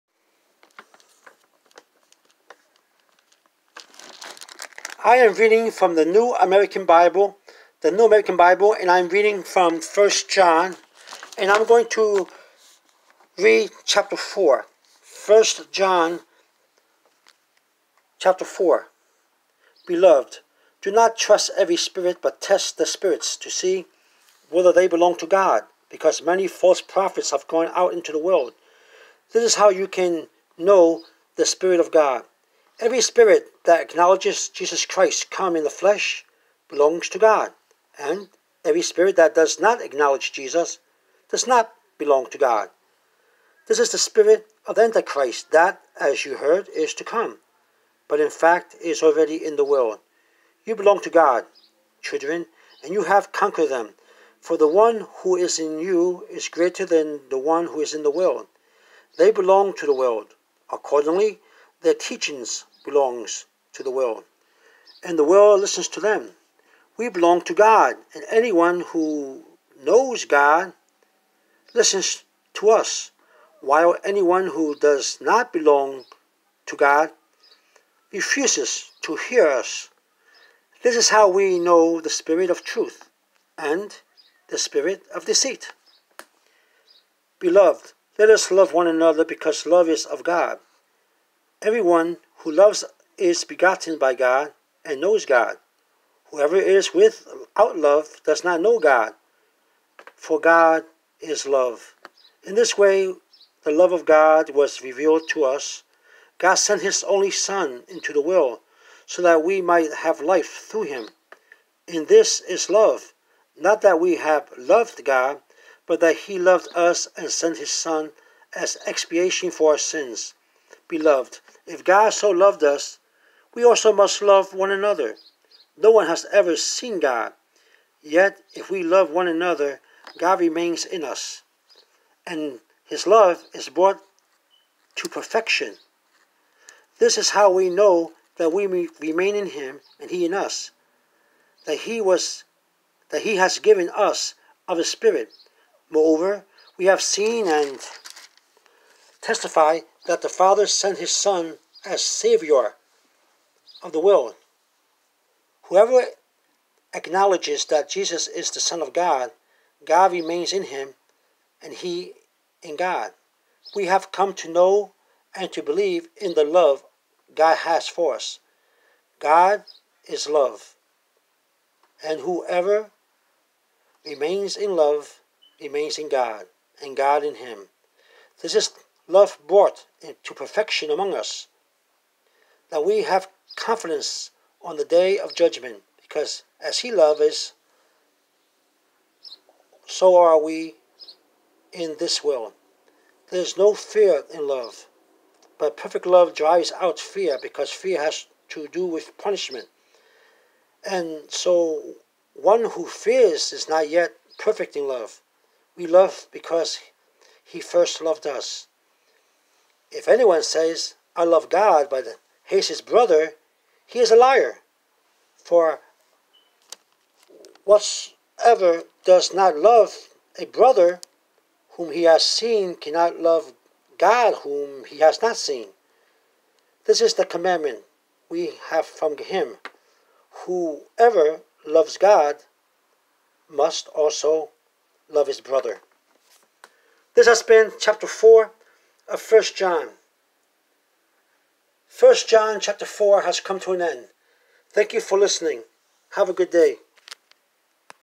The New American Bible. 1John chapter 4.
Testing the Spirits -- God's love and Christian life--  God is love.